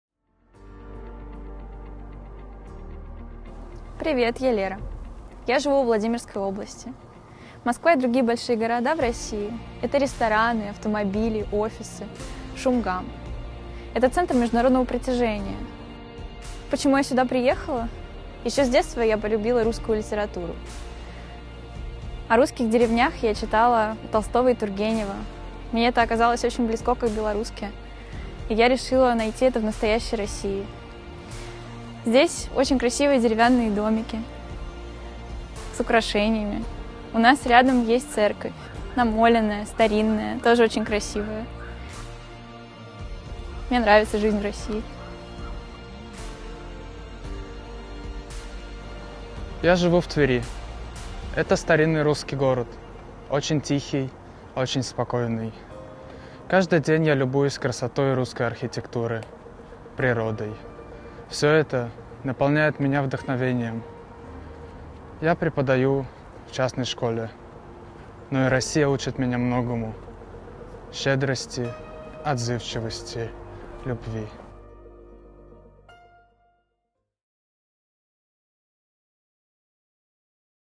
Я живу в настоящей России
Бескрайние горизонты, самобытная культура и гостеприимство людей покоряют сердца иностранцев, приезжающих в Россию. Узнав нашу страну лучше, некоторые находят в ней себя и остаются.